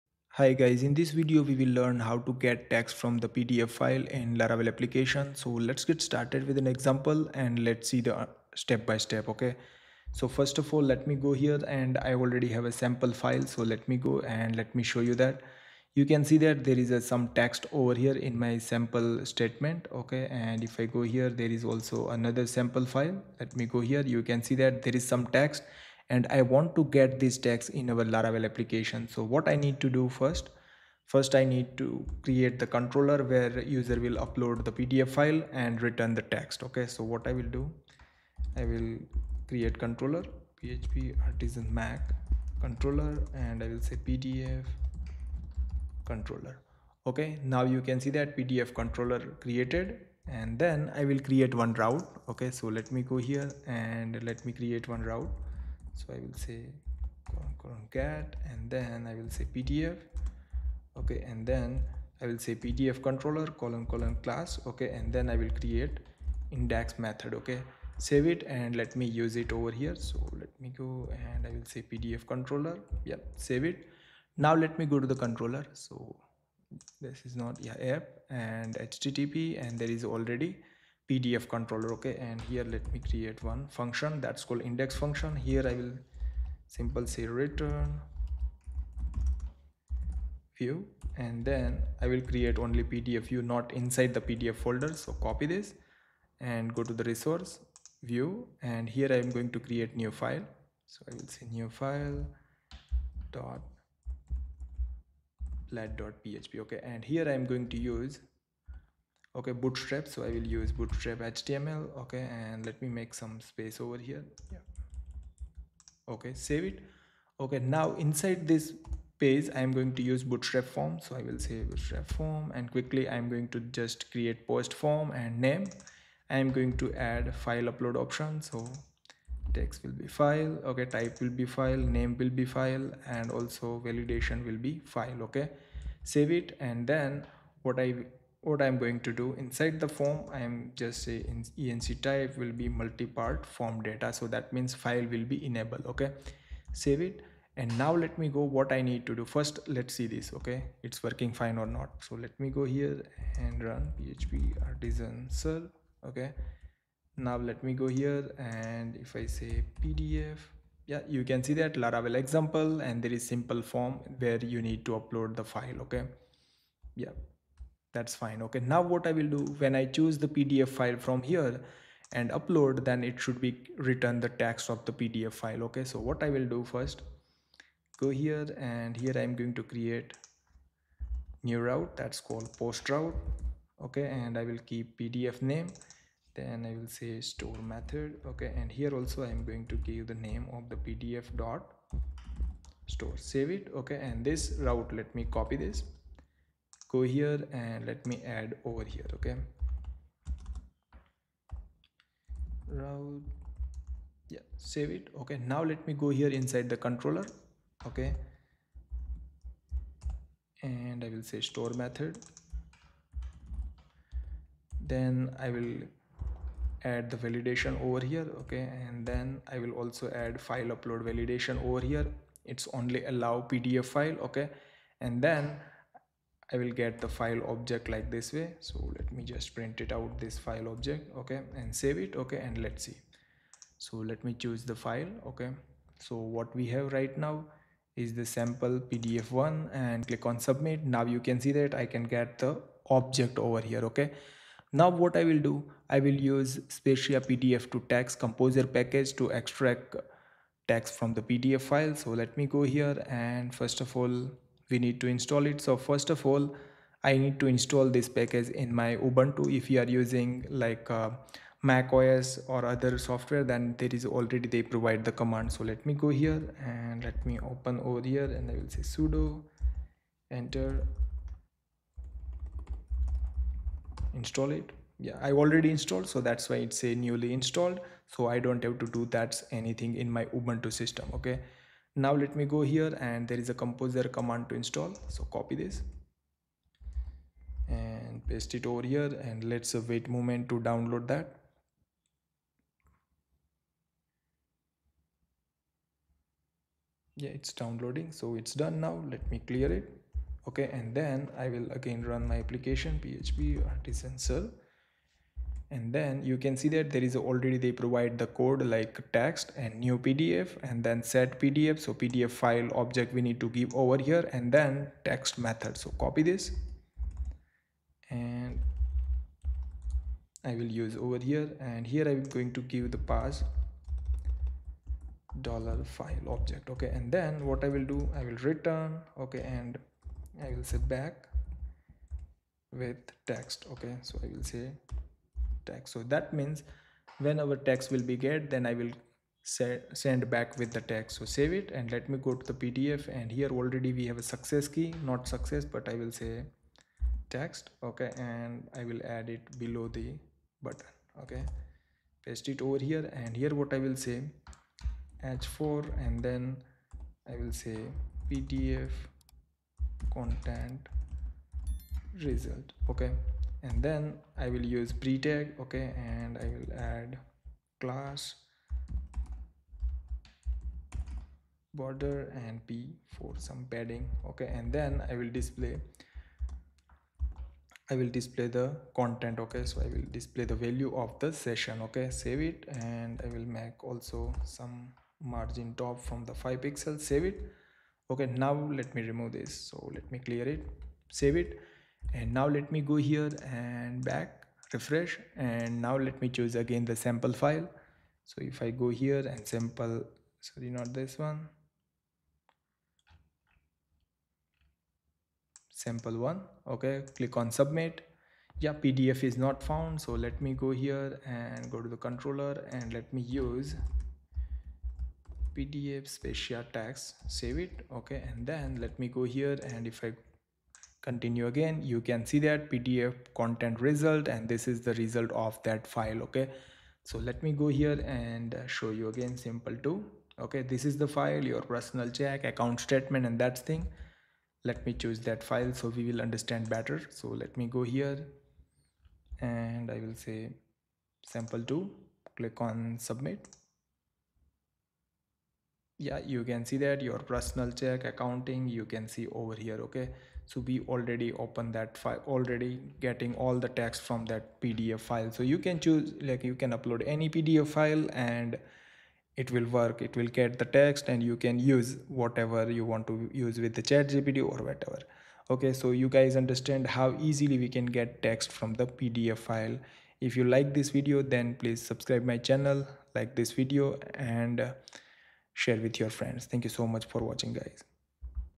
How to Convert PDF to Text in PHP Laravel
In this video, we will learn how to extract text from pdf file using spatie/pdf-to-text composer package in laravel.